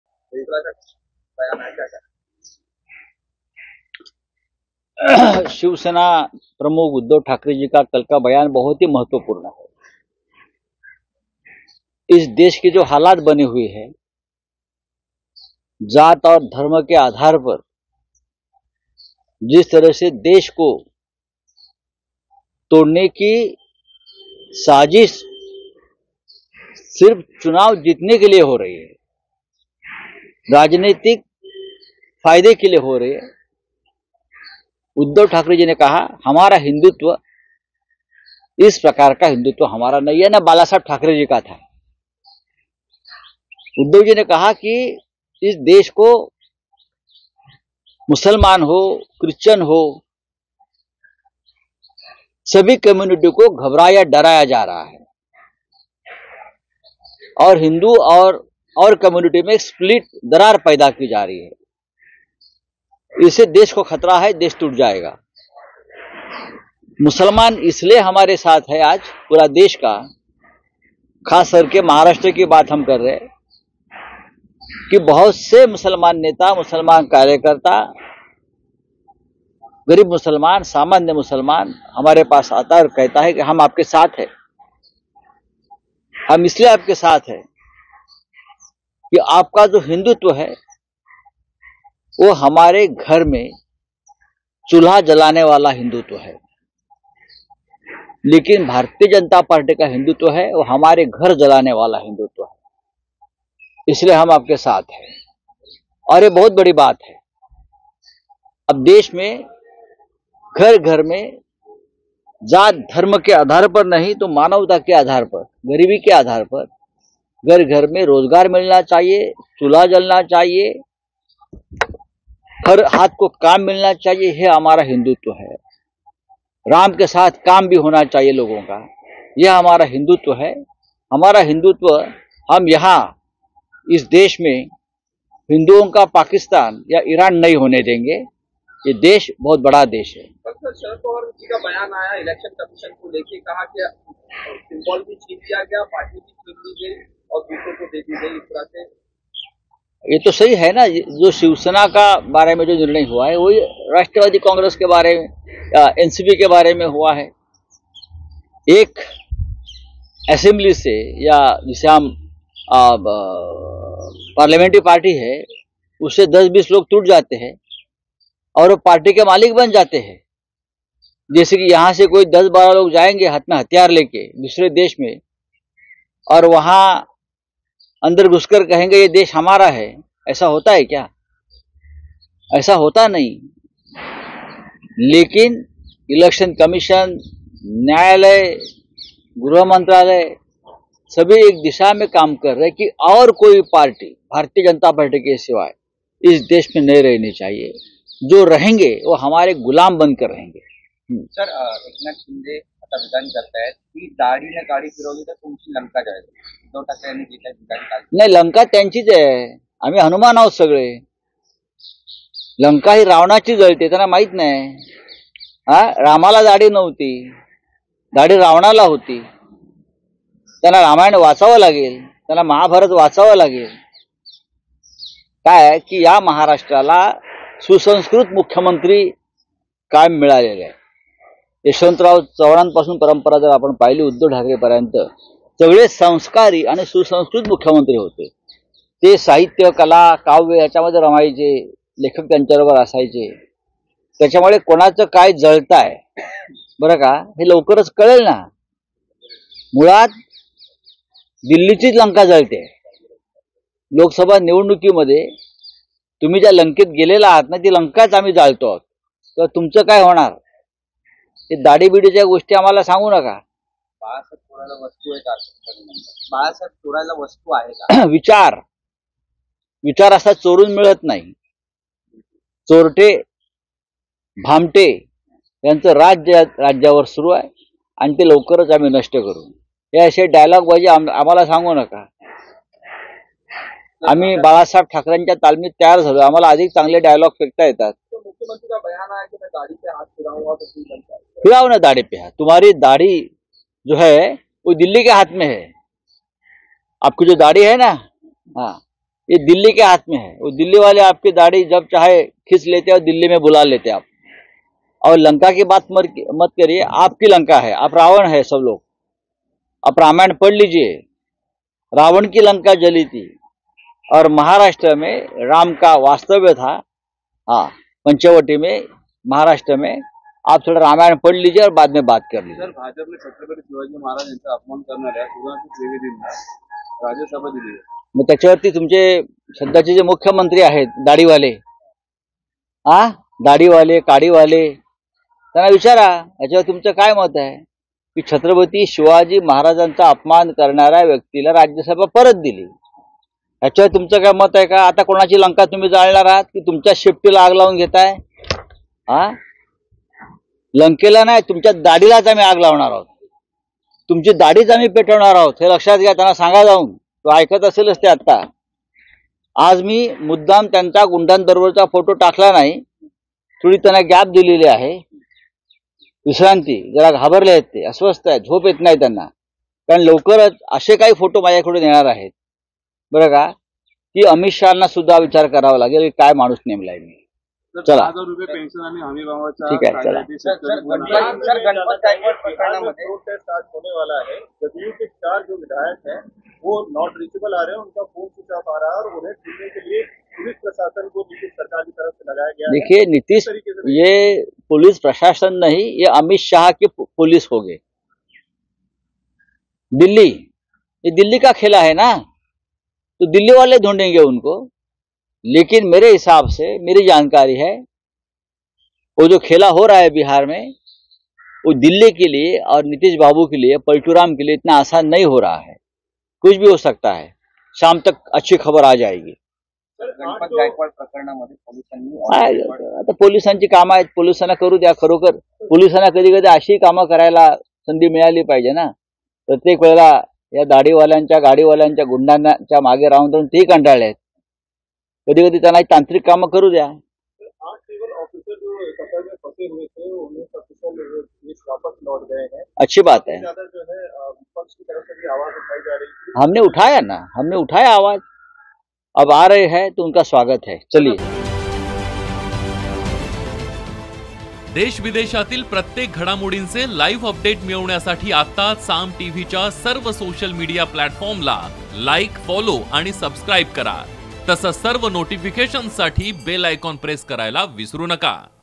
Sanjay Raut News : विचार असे चोरून मिळत नाही, राऊतांचा शिंदेंवर घणाघात
विचार असे चोरून मिळत नाही. चोरटे, भामटे यांचं राज्य सुरू आहे आणि आता लवकरच आम्ही ते नष्ट करू असे डायलॉगबाजी आम्हाला सांगू नका आम्ही बाळासाहेब ठाकरे यांच्या तालमीत तयार झाले आहोत आम्हाला अधिक चांगले डायलॉग करता येतात. अशी टीका राऊतांनी सरकारवर केली.| PKK |

Saam TV Marathi Live | Saam TV Marathi | Saam TV Live | Saam TV News | Marathi News | Marathi News Live | Marathi News Video